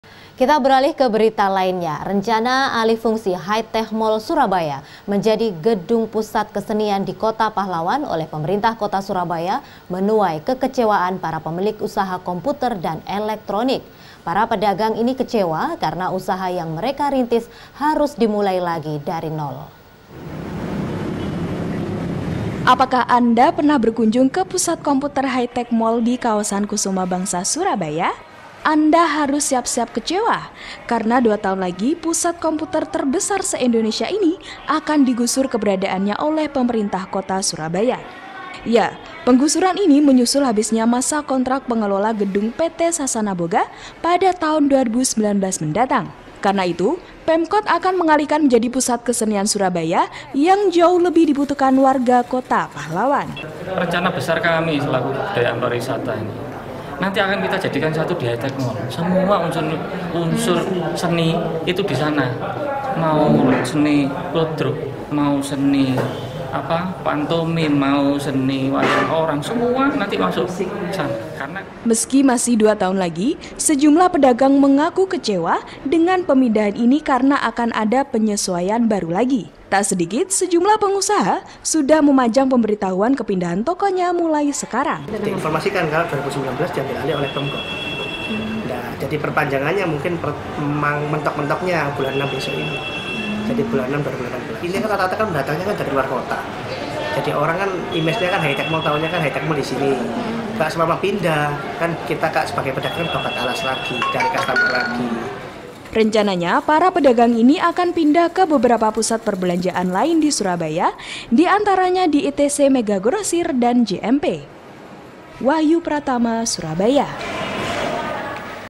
Alih Fungsi Hi Tech Mall
Rencana alih fungsi Hi Tech Mall Surabaya , menjadi gedung pusat kesenian di kota pahlawan, oleh Pemkot Surabaya menuai kekecewaan para pemilik usaha komputer dan elektronik. Para pedagang eletronik ini, kecewa karena harus mencari pelanggan lagi di tempat yang baru.

Like & Follow Facebook:
CNN Indonesia

cnnindonesia